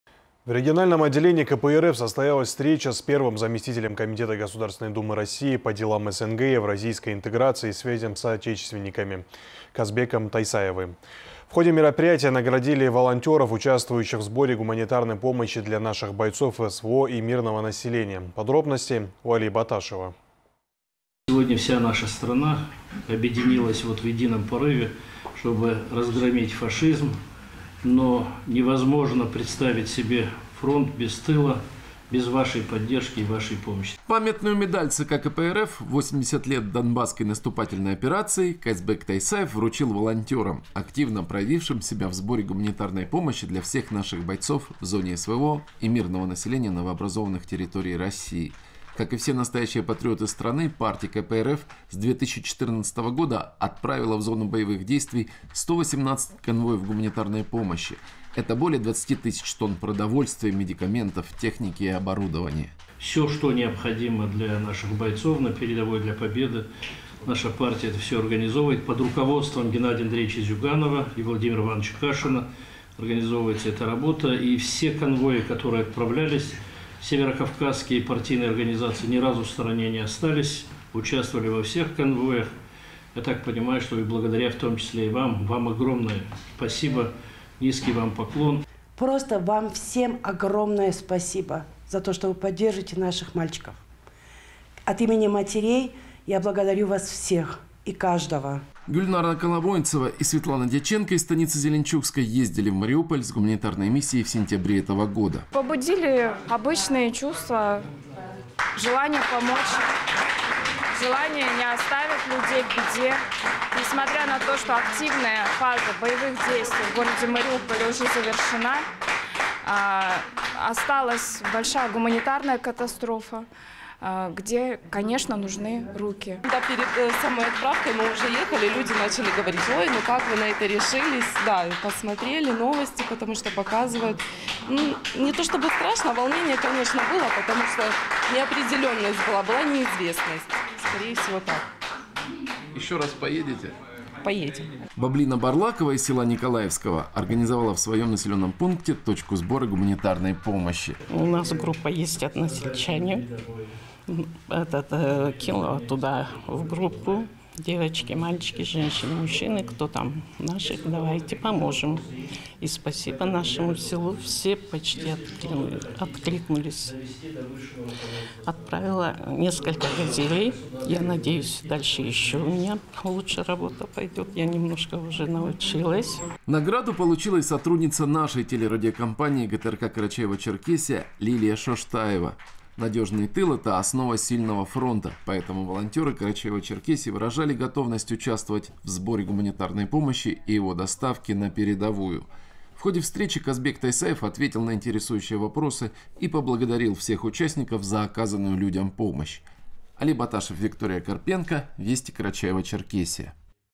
В региональном отделении КПРФ состоялась встреча с Казбеком Тайсаевым
В региональном отделении КПРФ состоялась встреча с первым заместителем комитета Государственной Думы России по делам СНГ и Евразийской интеграции, связям с соотечественниками Казбеком Тайсаевым. В ходе мероприятия наградили волонтеров, участвующих в сборе гуманитарной помощи для наших бойцов в СВО и мирного населения